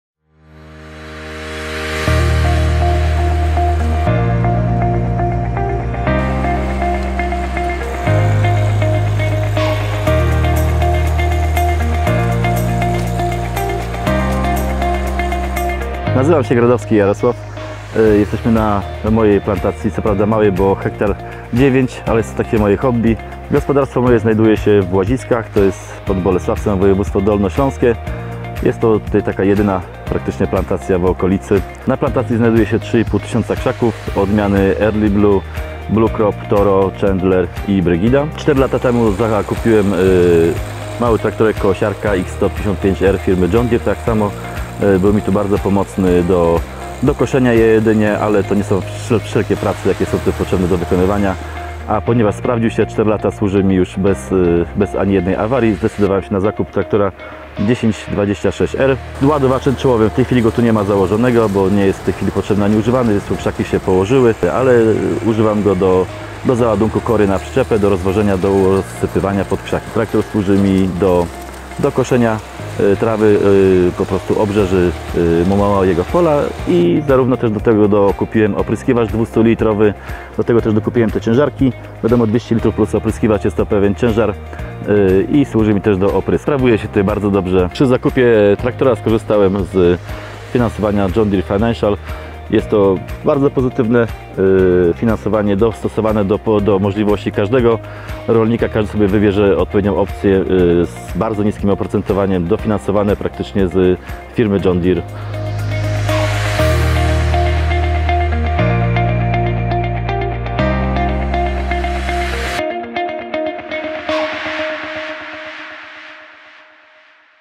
Kompaktowy ciągnik serii 1 na plantacji borówki | John Deere Financial
Plantacja borówki i kompaktowy John Deere 1026? To doskonałe połączenie! Zobacz jak pracuje ciągnik kompaktowy John Deere na borówkowej plantacji naszego Klienta – Jarosława Gradowskiego. Maszyna została zakupiona w finansowaniu fabrycznym 5 lat 0% John Deere Financial. Szczegóły oferty